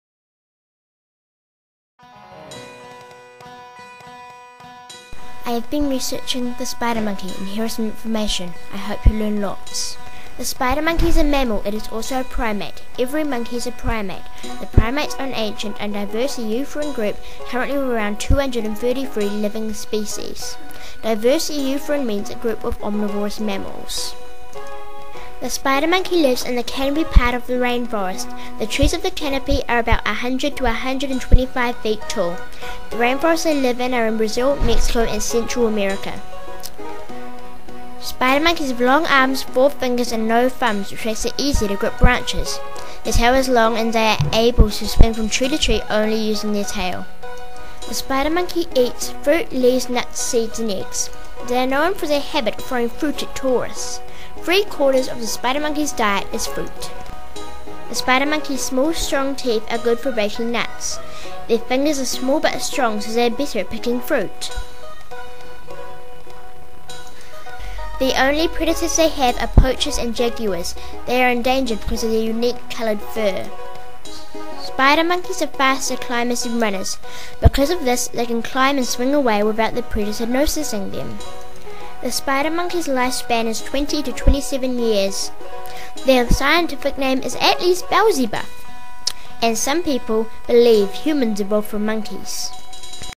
Melanie's Spider Monkey Documentary
A short documentary about the spider monkey created by a Year 6 student using photostory3.